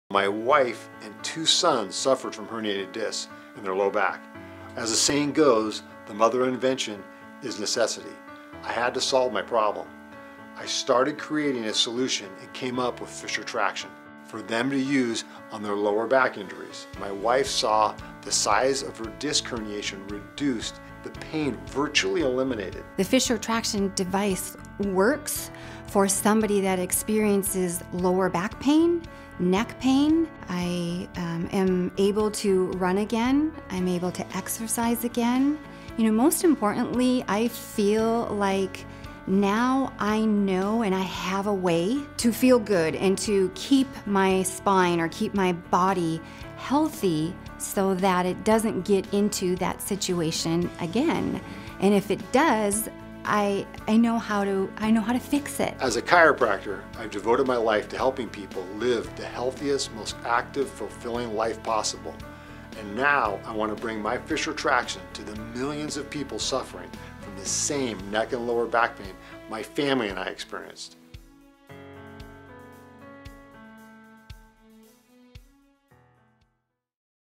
Best home traction for the neck and low back pain.
Dr. Fisher invented and patented this new revolutionary type of traction that you can use virtually anywhere. All you need is a door knob and some floor space, it's as easy as lying down! Decompress your spine at home with the same force as the expensive tables in chiropractor's offices. Very inexpensive and easy to use. Weighs less than a pound so you can travel with it anywhere. Has as much pull force for people who weigh up to 320 lbs. Dr. Fisher is a chiropractor from California who created this for himself and his family 20 years ago, only recently has he released it to the public after years of perfecting his invention to insure safety and ease of use. learn more